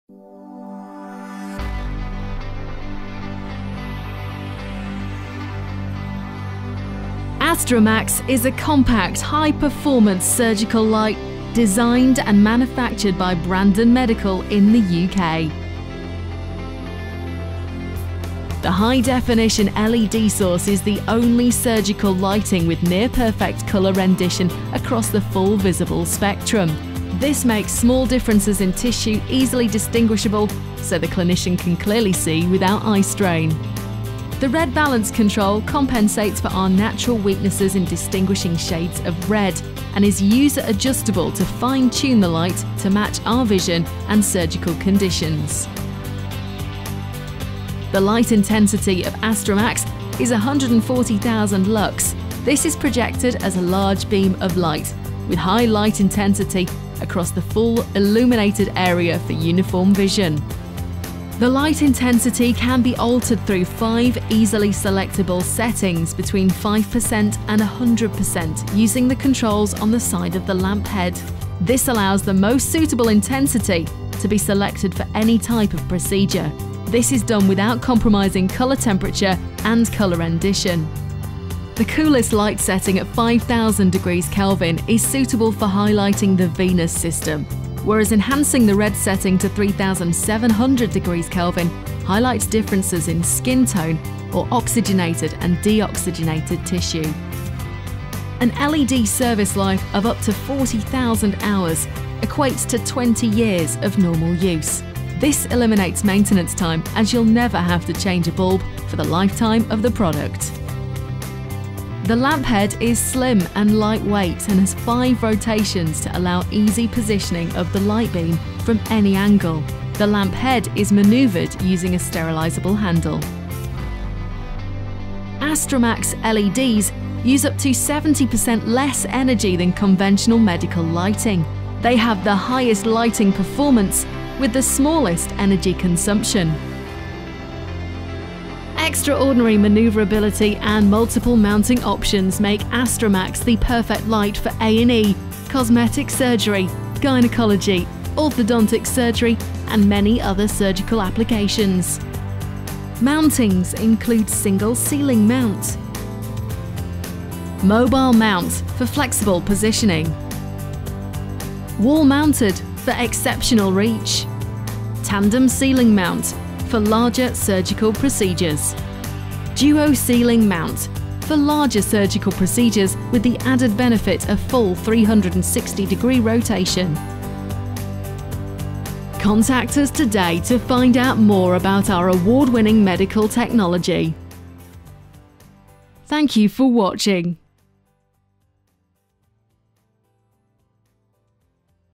Astramax HD-LED surgical light by Brandon Medical
Astramax HD-LED is a high performance, compact surgical light designed and manufactured by Brandon Medical. HD-LED provides the only surgical lighting technology with near pefect colour rendition across the full visible spectrum.